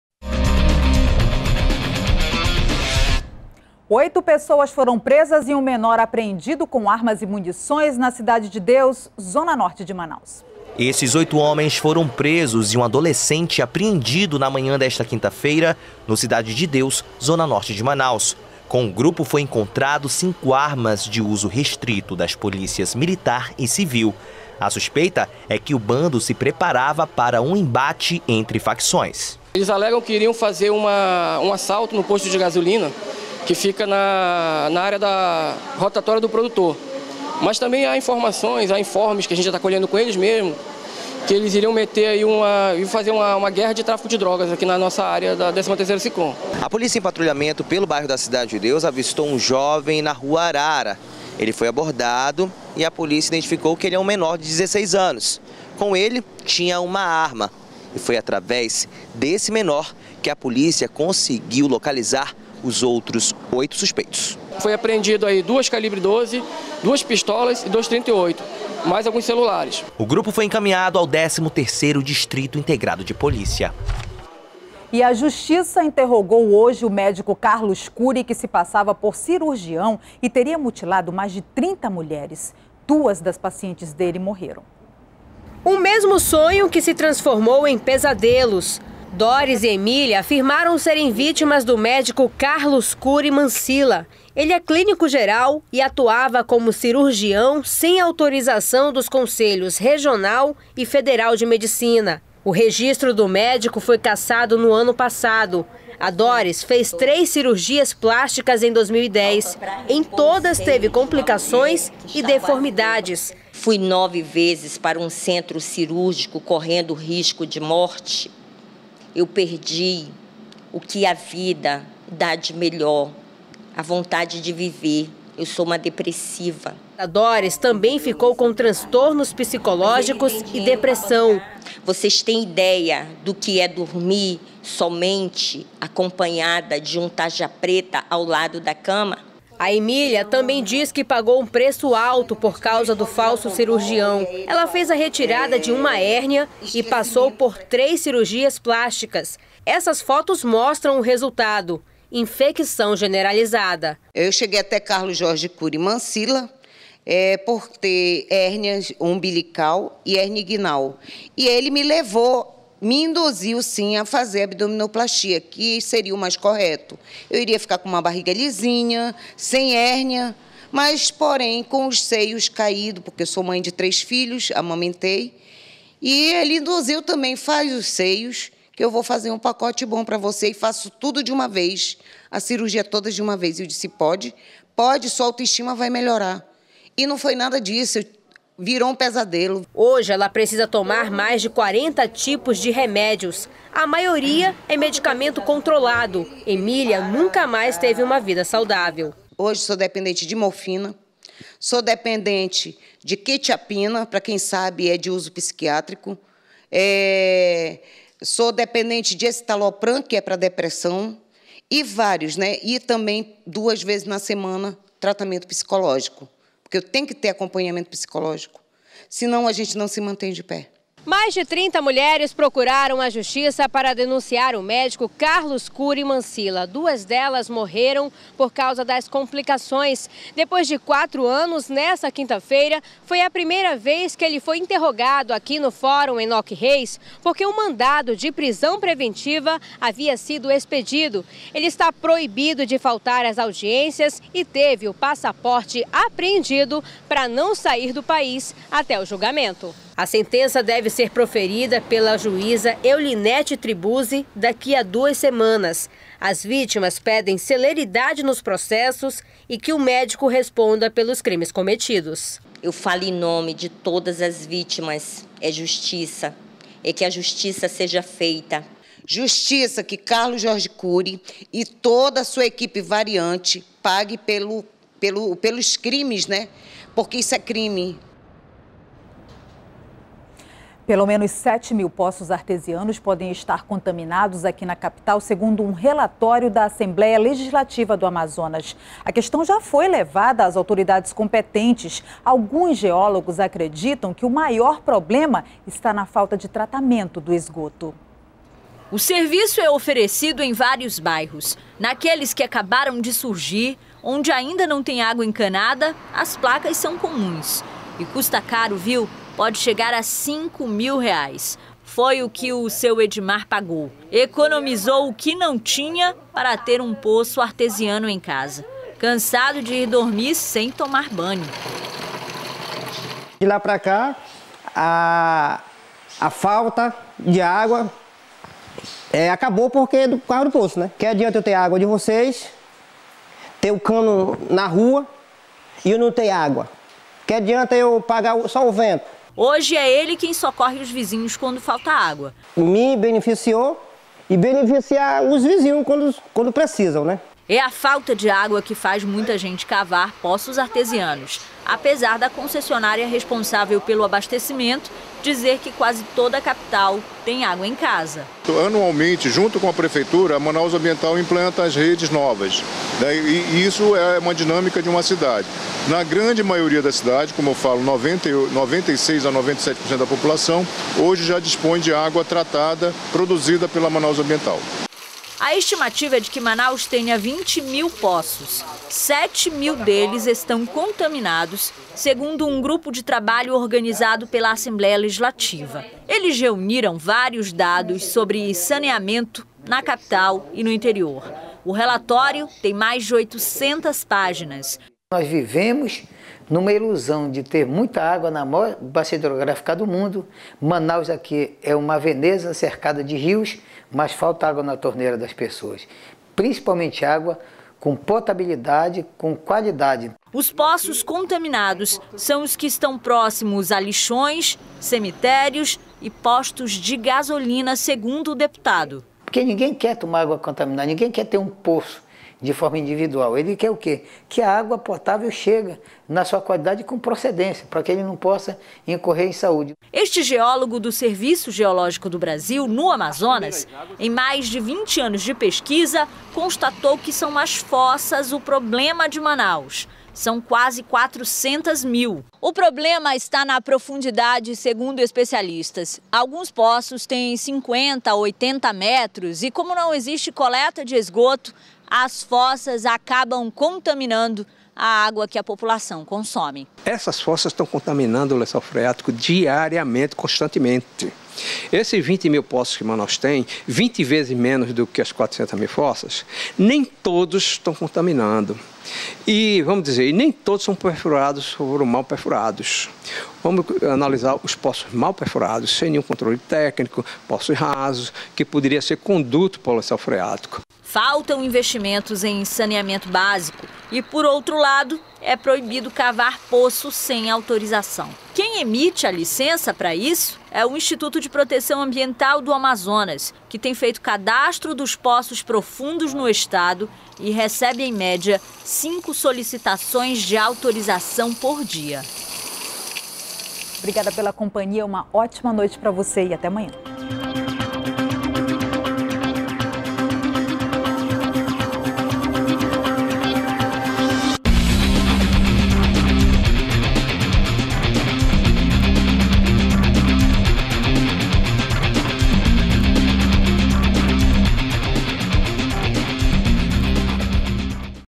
Justiça ouve pela primeira vez o médico acusado de mutilar mulheres em cirurgias aqui no estado
03º BLOCO JORNAL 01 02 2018 - Justiça ouve pela primeira vez o médico acusado de mutilar mulheres em cirurgias aqui no estado